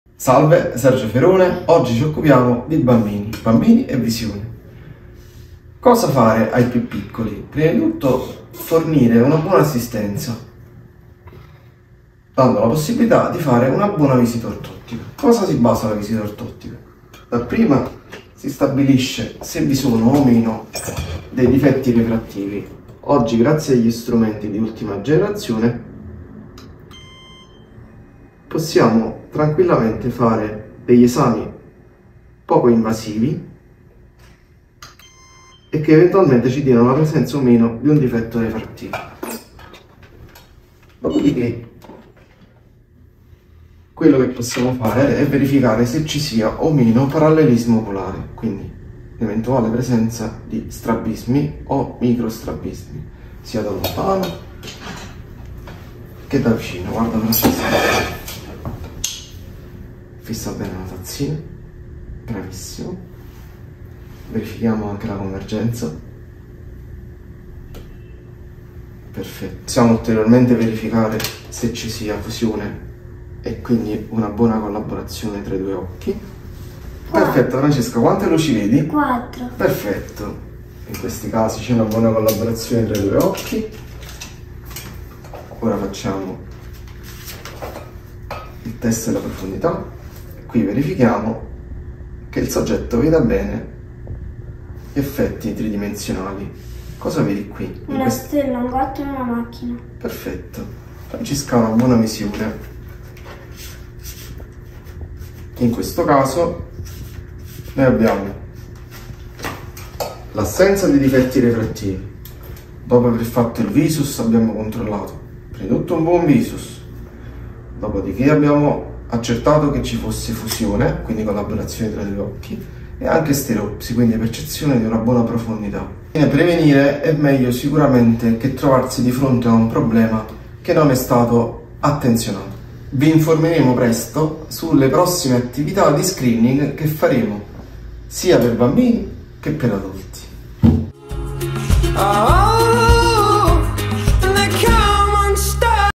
Ortottista Napoli - Sergio Ferone Visione e Bambini
Nel video di oggi parliamo dell'importanza della prevenzione visiva nei bambini e di come una visita ortottica preventiva possa fare la differenza nella loro salute oculare. Spesso i problemi di vista nei più piccoli sono difficili da individuare, soprattutto perché molti bambini non sono in grado di esprimere chiaramente ciò che non vedono correttamente.

La visita ortottica è un controllo fondamentale per identificare tempestivamente eventuali disturbi visivi come strabismo, ambliopia (occhio pigro), difficoltà di convergenza e altri problemi che possono influire sullo sviluppo della vista e sulla qualità della vita quotidiana del bambino.

Durante la visita ortottica, il professionista utilizza tecniche specifiche per valutare la motilità oculare, la coordinazione tra i due occhi, e la presenza di eventuali anomalie nella visione. Intervenire precocemente consente di correggere molti di questi problemi con trattamenti efficaci e, spesso, non invasivi.

Perché è importante fare una visita ortottica preventiva?

Prevenzione di problemi futuri: risolvere tempestivamente i disturbi visivi può evitare complicazioni a lungo termine.
Miglioramento delle performance scolastiche: una buona vista è cruciale per il successo in ambito educativo.
Qualità della vita: una diagnosi precoce permette al bambino di vivere senza limitazioni visive, migliorando la sua sicurezza e autostima.
Non aspettare che il problema si manifesti! Se noti che tuo figlio ha difficoltà a leggere, concentrarsi o se ti sembra che abbia spesso mal di testa, potrebbe essere il momento giusto per una visita ortottica. La prevenzione è il primo passo per garantire una crescita sana e un futuro luminoso!

💬 Scrivici nei commenti se hai domande o esperienze da condividere sulla salute oculare dei tuoi bambini.